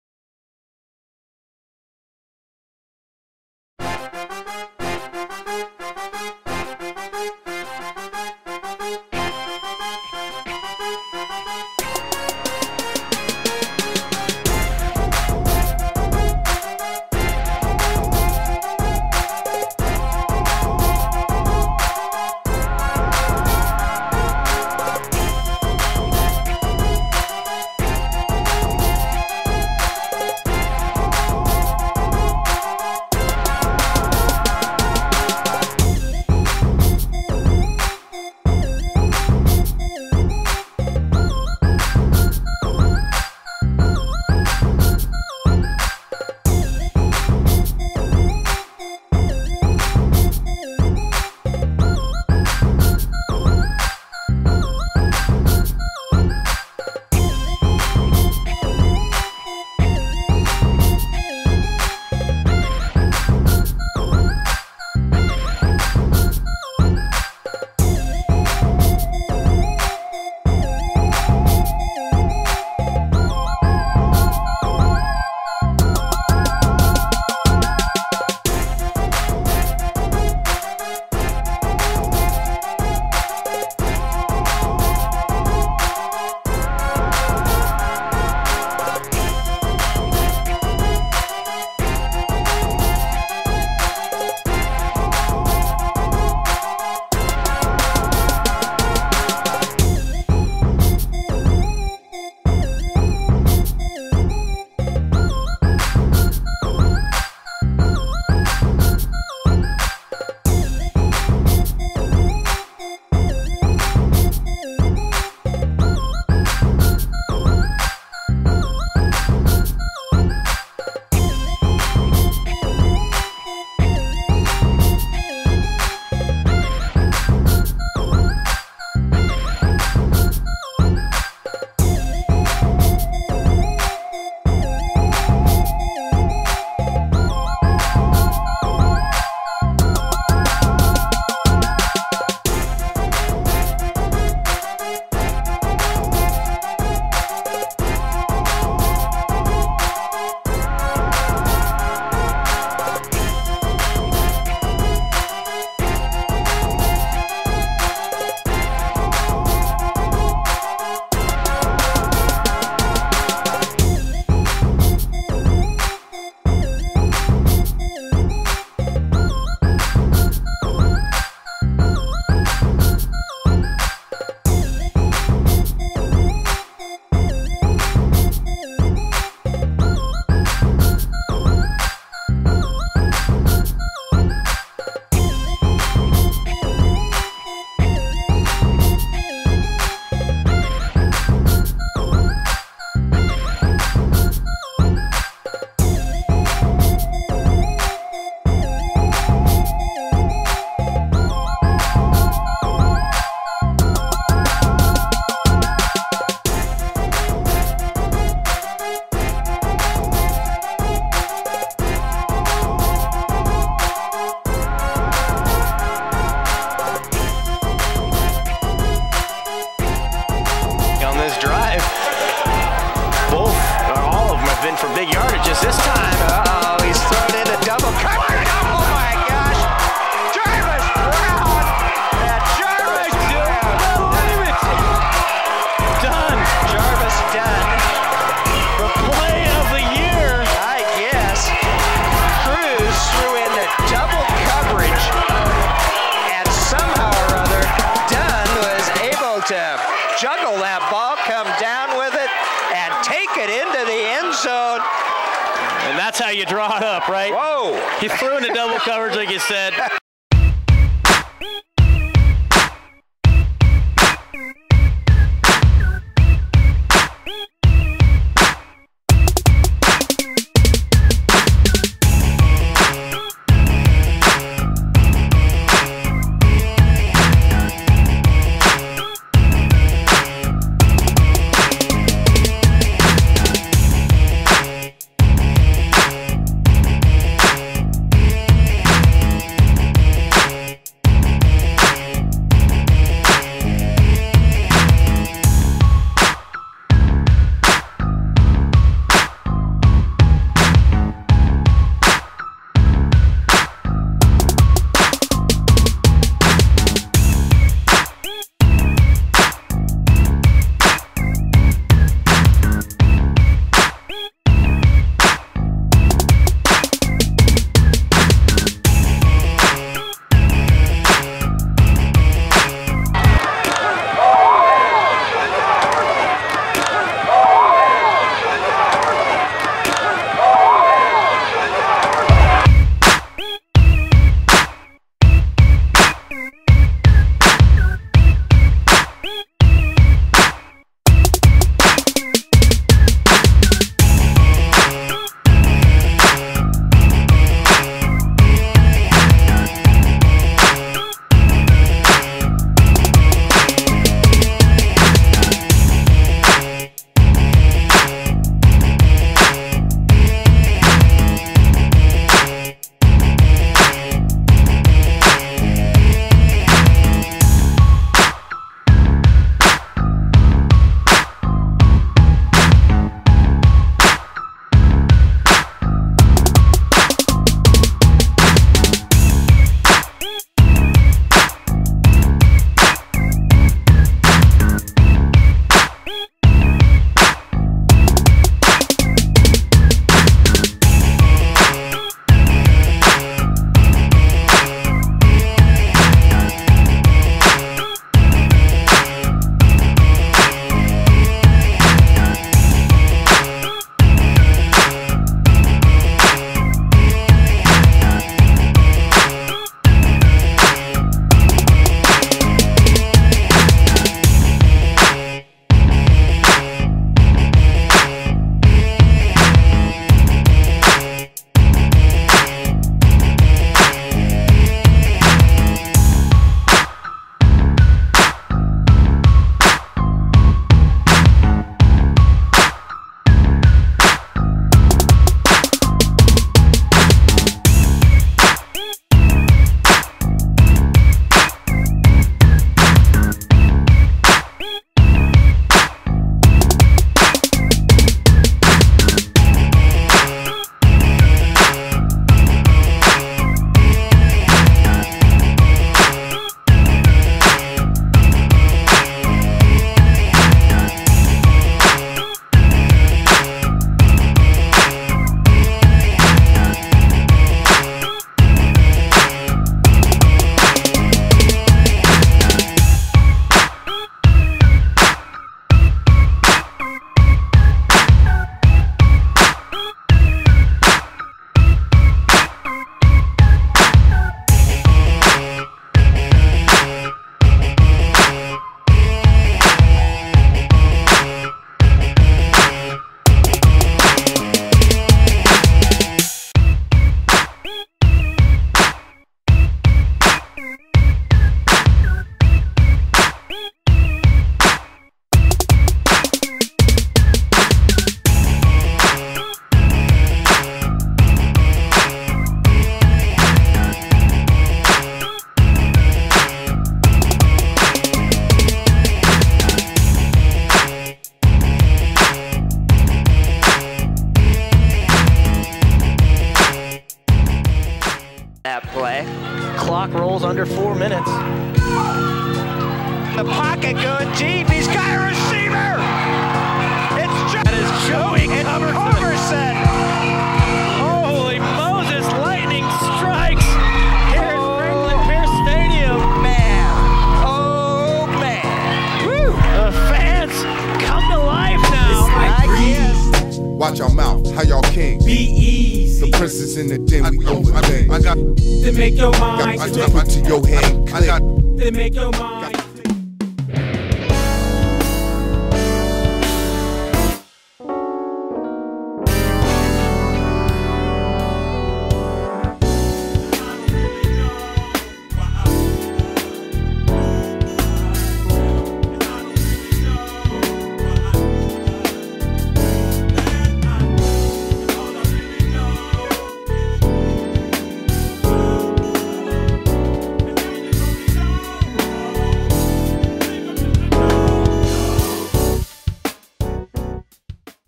PDFL CHAMPIONSHIP
2009 PDFL CHAMPIONSHIP GAME TACOMA COBRAS VS BELLINGHAM BLITZ INWHICH THE TACOMA COBRAS WON A THRILLER IN TACOMA. COBRAS WIN THE 1ST EVER TOM ABBOTT TROPHY